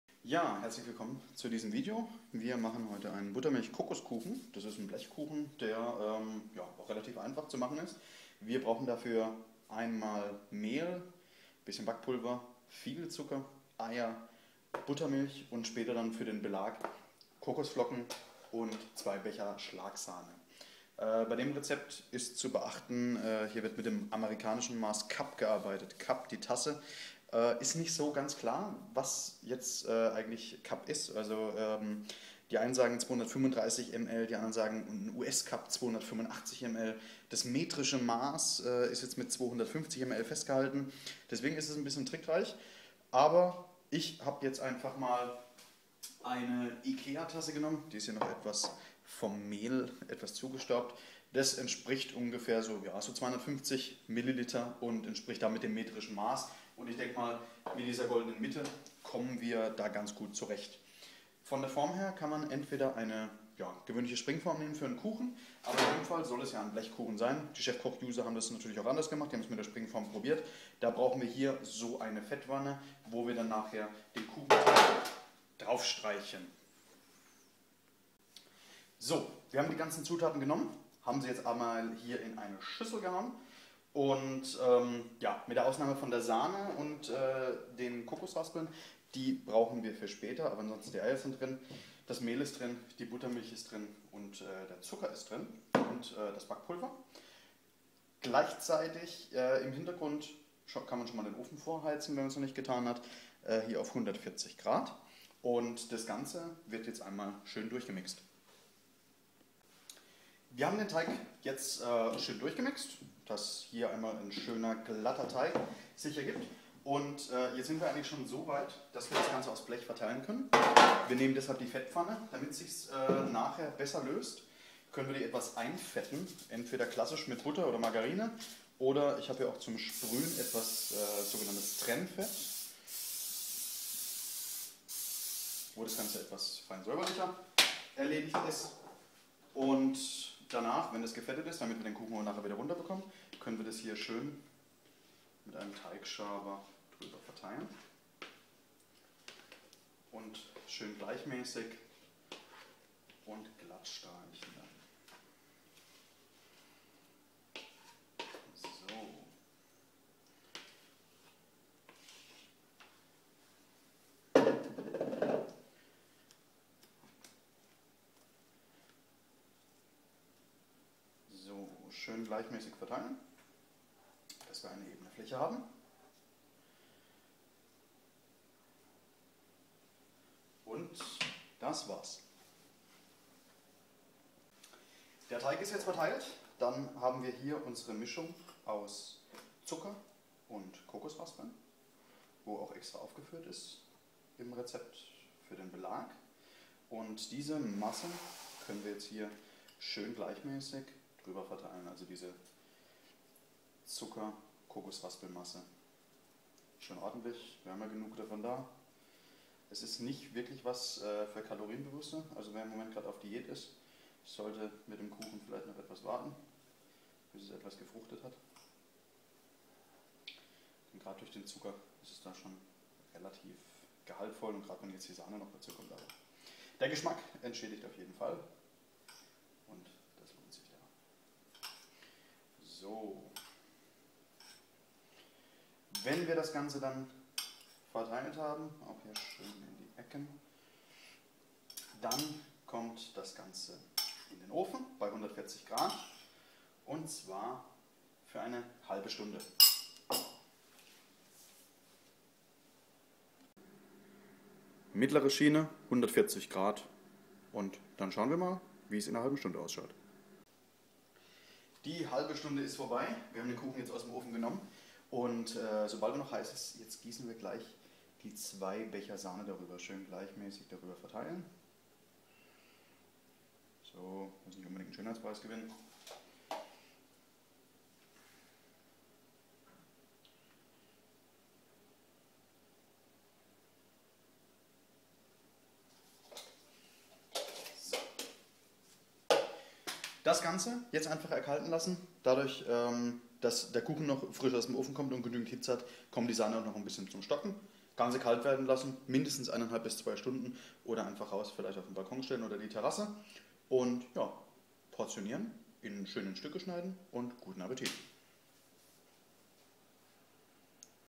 Rezept: Buttermilch - Kokos - Kuchen backen/ Buttermlich Kokos Kuchen zubereiten/ Back anleitung
Das beste Buttermilch - Kokos - Kuchen backen Video auf YouTube. Wenn euch das Video gefällt, dann liked es doch einfach.
Wer Buttermilch - Kokos - Kuchen backen möchte, muss nur folgende Zutaten wie im Video benutzen:  zwei Tassen Butter - Milch und Zucker, drei Eier, vier Tassen Mehl und anderthalb Päckchen Backpulver. Außerdem für den Belag zwei Tassen Kokosraspeln, eine Tasse Zucker und zwei Becher süße Sahne. So kann man aus einfachen Zutaten wie Buttermilch, Zucker, Eier, Butter - Milch - Kokos - Kuchen backen selber machen.